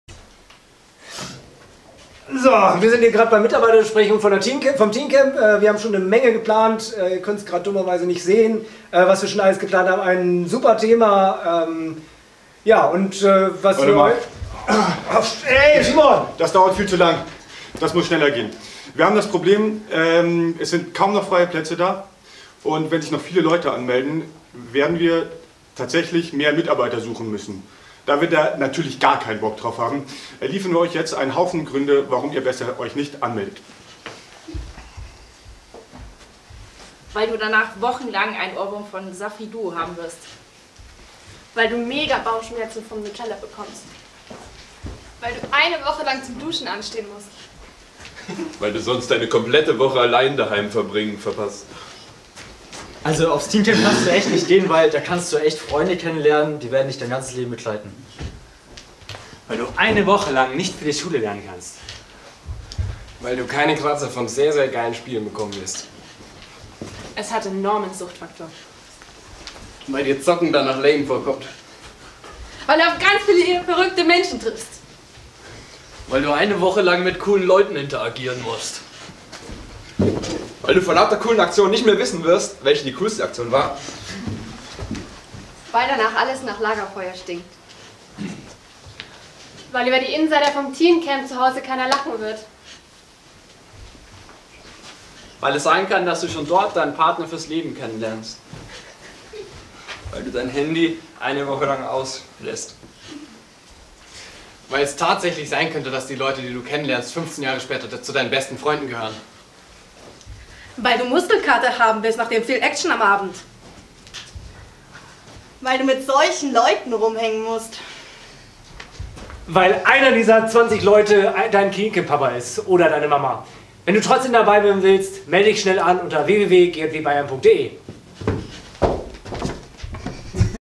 Teencamp 2015 - Gründe dagegen
Du bist dir nicht sicher, ob du zum Teencamp mitfahren sollst? Wir liefern dir viele Gründe dagegen.